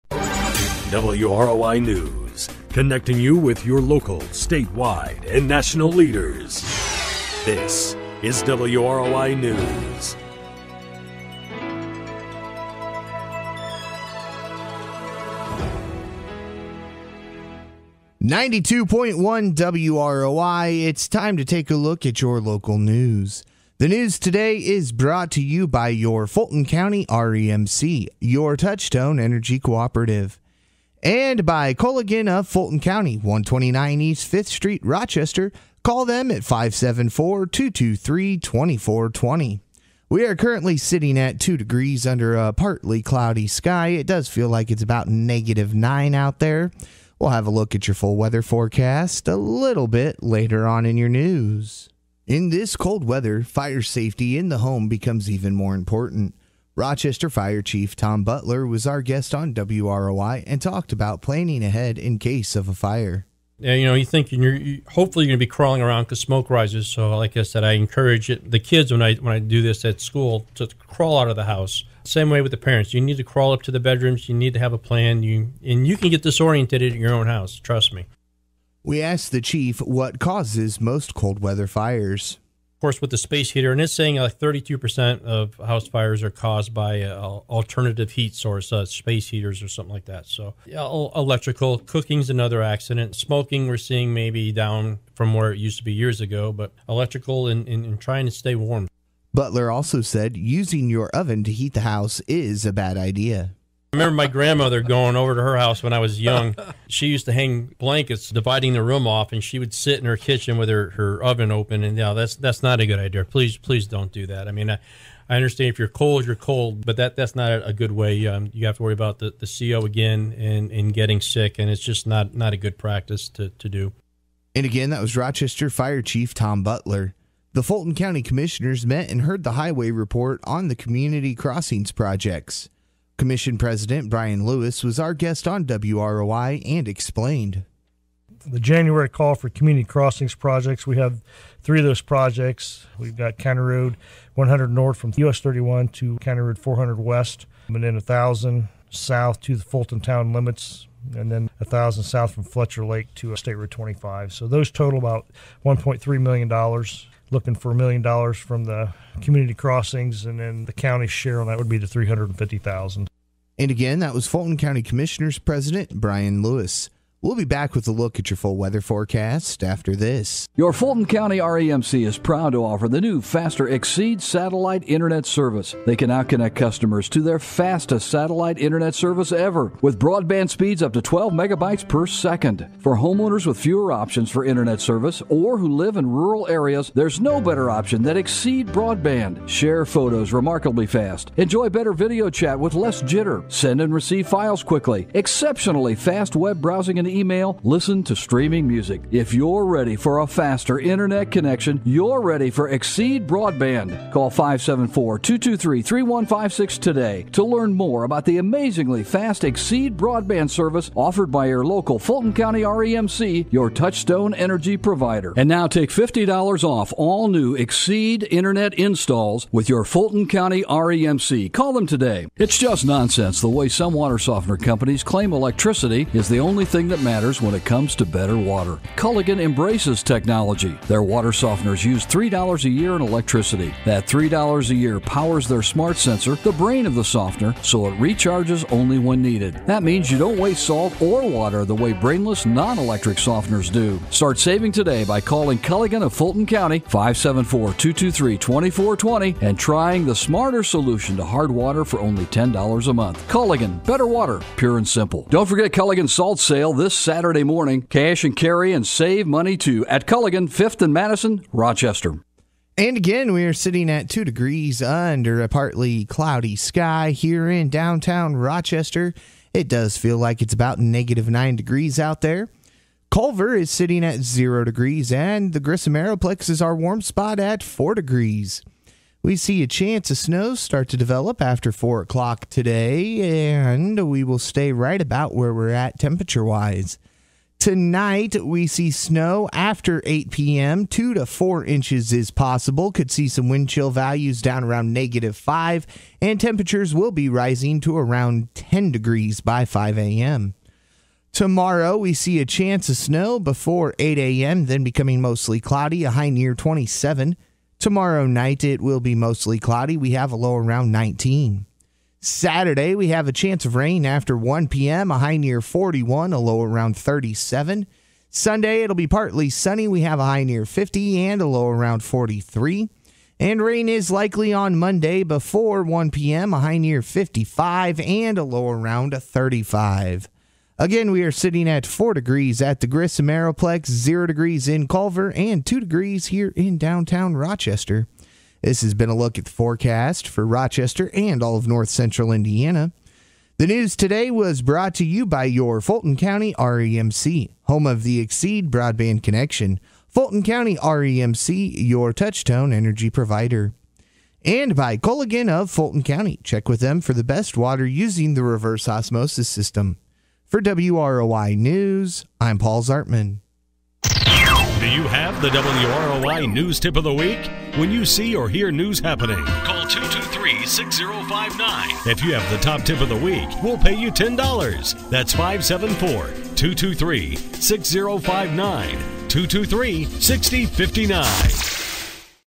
Thursday January 31, 2019
Today we continued talking with Fire Chief Tom Butler about the cold and fire hazards.  Also heard from Fulton County Commissioner Brian Lewis about some of the County Crossings Projects.  Looked at that much warmer forecast.